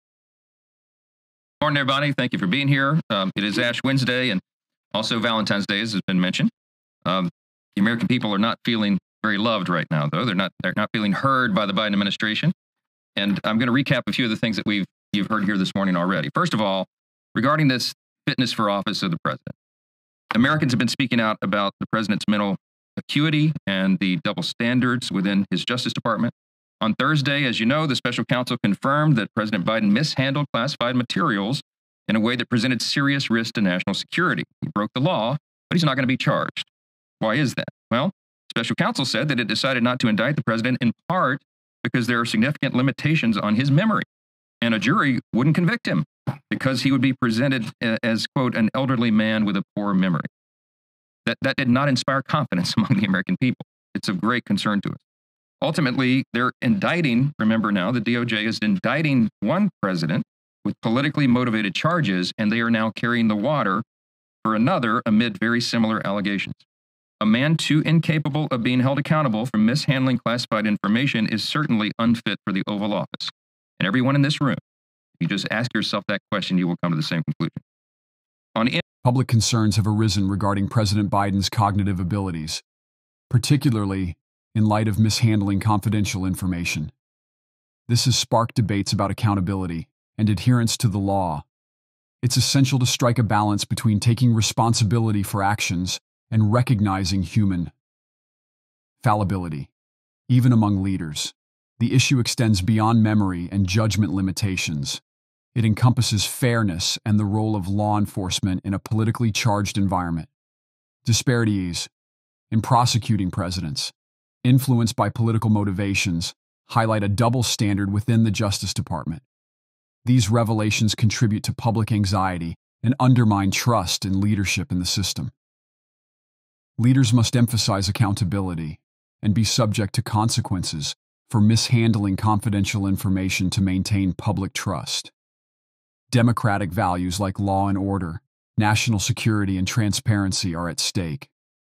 Speaker Mike Johnson, Biden's Mishandling of Classified Information
In this exclusive video, Speaker Johnson sheds light on President Biden's mishandling of classified information, sparking widespread concern about his fitness for office.